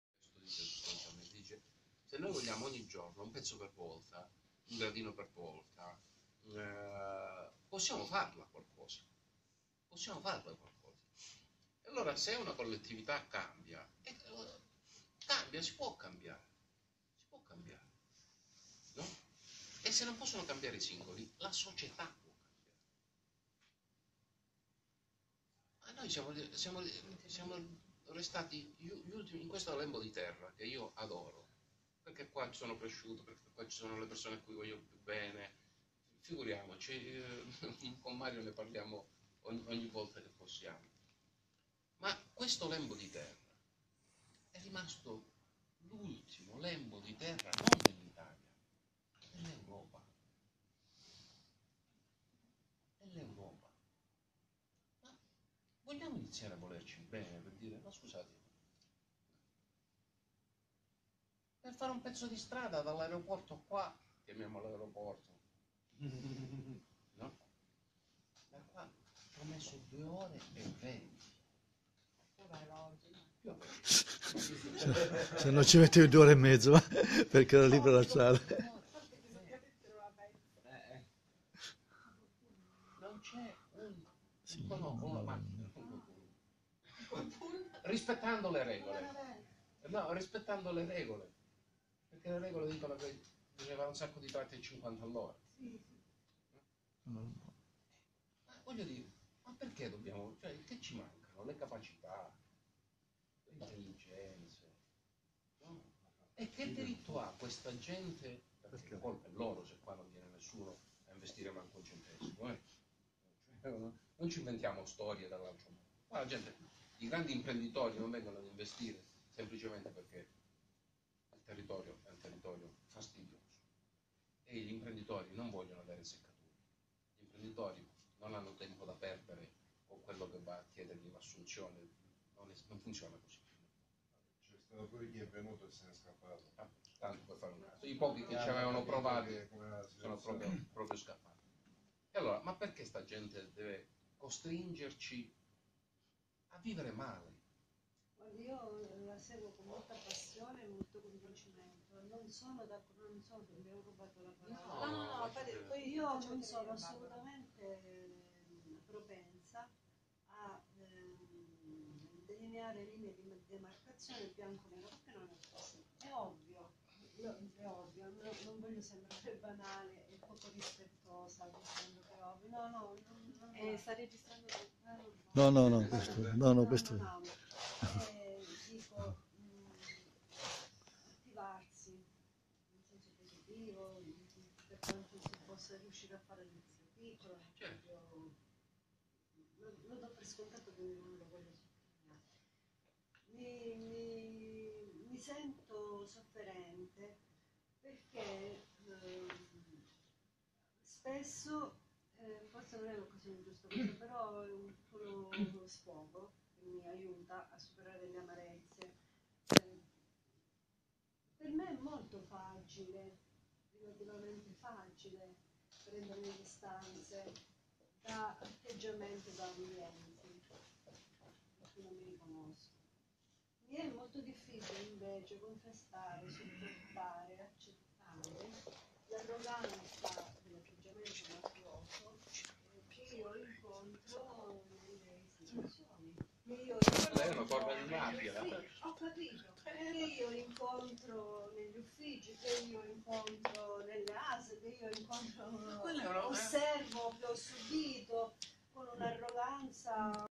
Giuseppe Baldessarro
Presentazione del libro "Questione di rispetto"  alla libreria Mondadori di Siderno